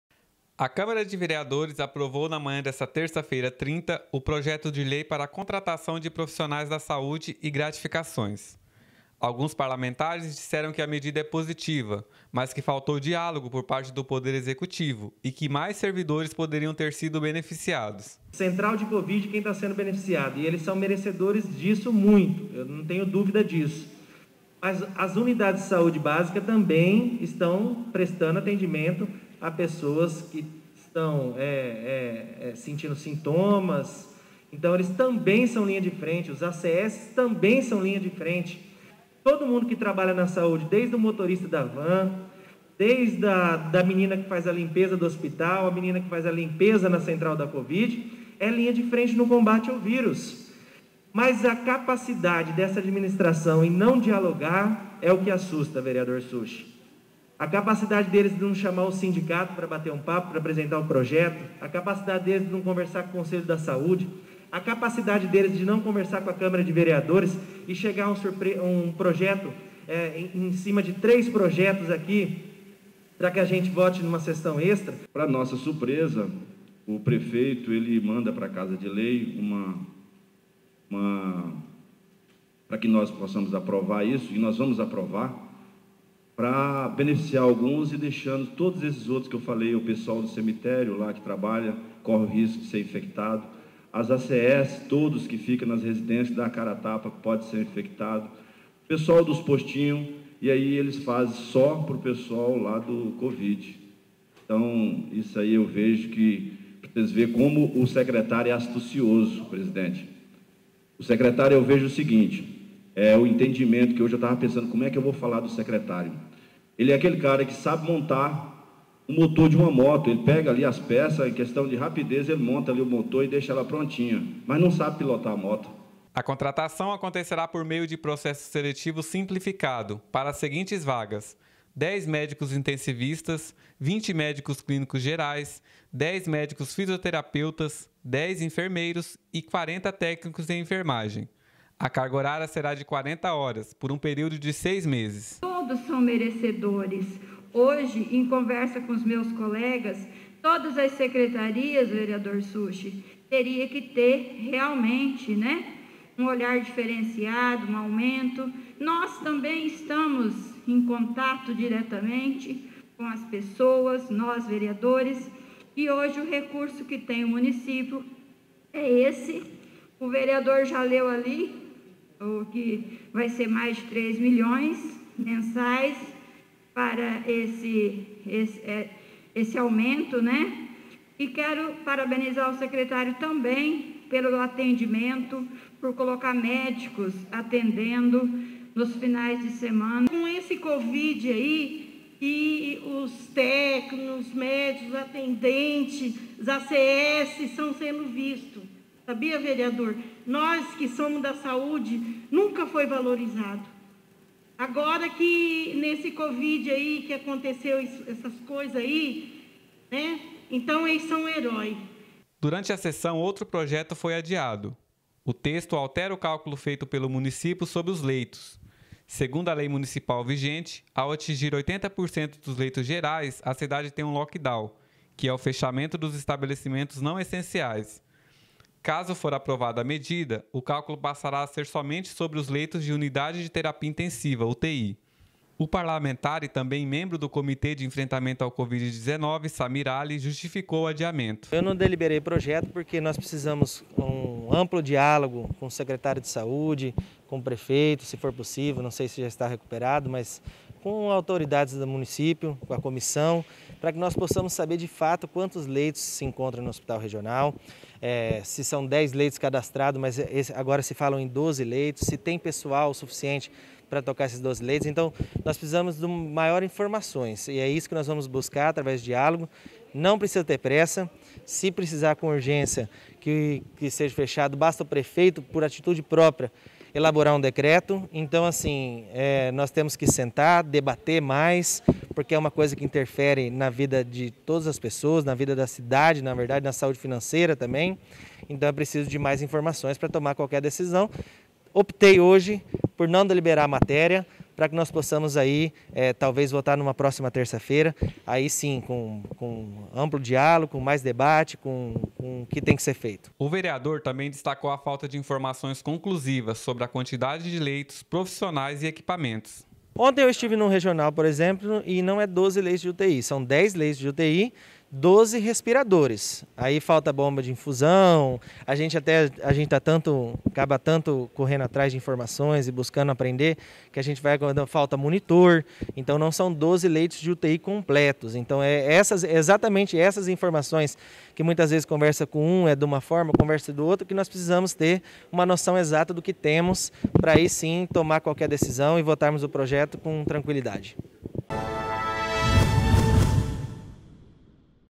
VEREADORES APROVAM LEI PARA INSTALAÇÃO DE SUBESTAÇÃO NO PRÉDIO DO VERDURÃO E CRÉDITO ADICIONAL
REPÓRTER: JOEL JONAS
IMAGENS: FLÁVIO GOMES
EDIÇÃO: EVERTON MELLO